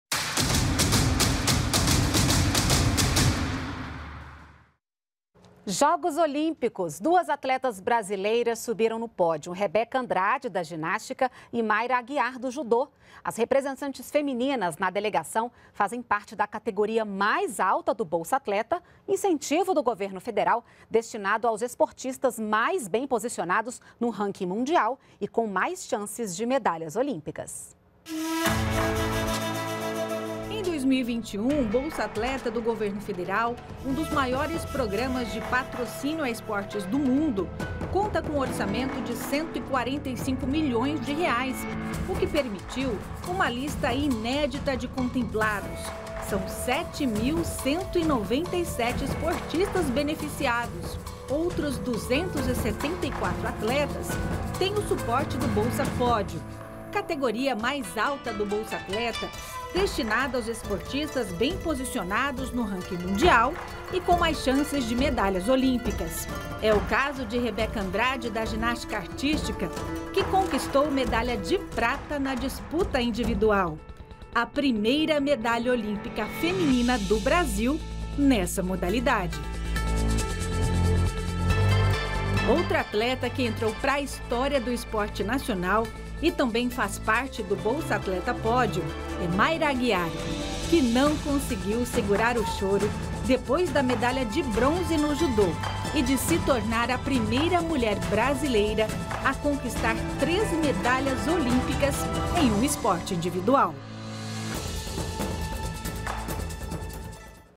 Jogos Olímpicos de Tóquio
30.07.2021 –Duas atletas brasileiras subiram no pódio: Rebeca Andrade, da ginástica, e Mayra Aguiar, do judô. As representantes femininas na delegação fazem parte da categoria mais alta do Bolsa Atleta, incentivo do governo federal destinado aos esportistas mais bem posicionados no ranking mundial e com mais chances de medalhas olímpicas.